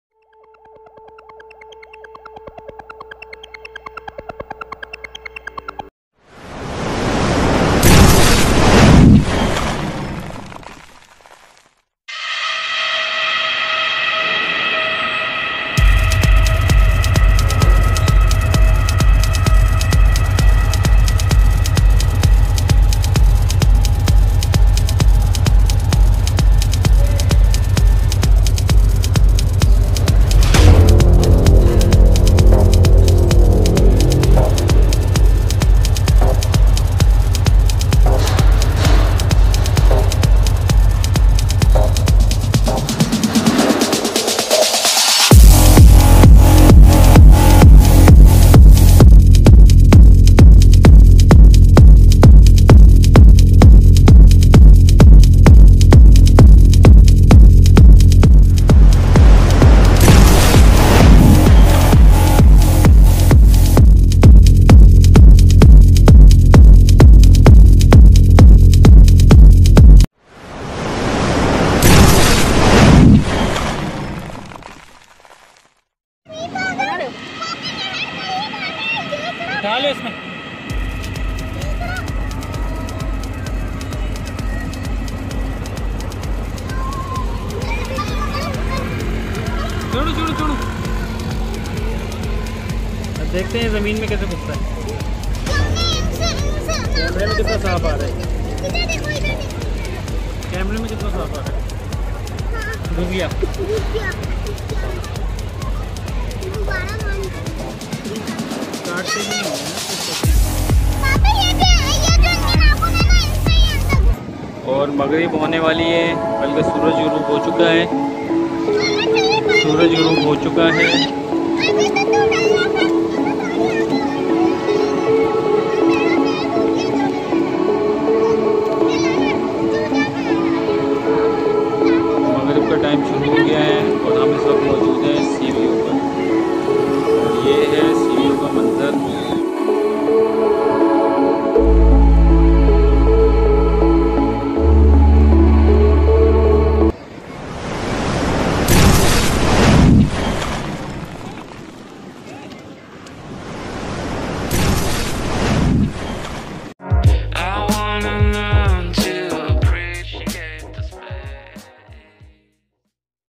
sea view ki matti me kiya nikal raha hai aj kal..allert rahen
plz picnic jane wale apne bacho ka kheyal rakhen..